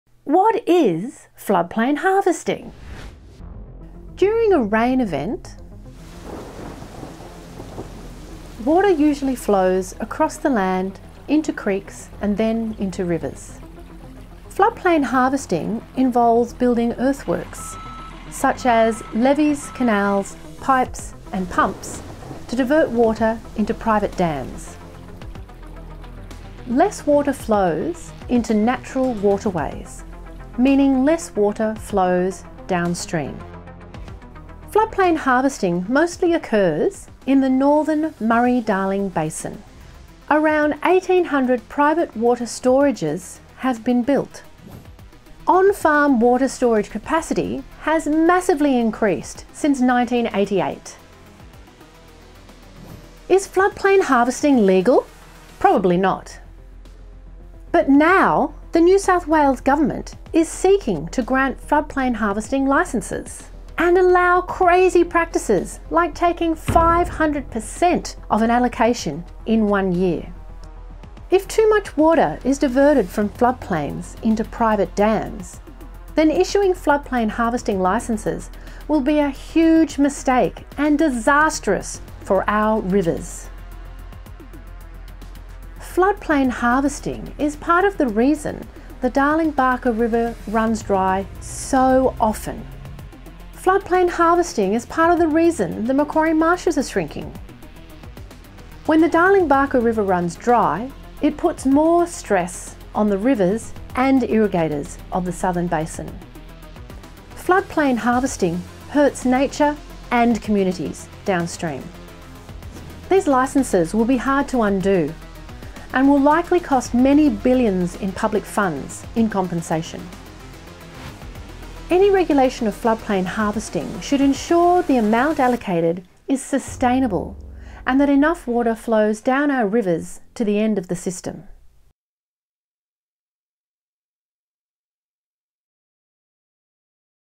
Floodplain Harvesting Explainer
What is floodplain harvesting? Why is everybody so worried about its impact on rivers and people downstream?
Watch this video to understand why we are concerned that the NSW Government is about to issue a new type of water licences with massive implications for our rivers and wetlands.
Are we about to make the same mistake of the past and overallocate water resources?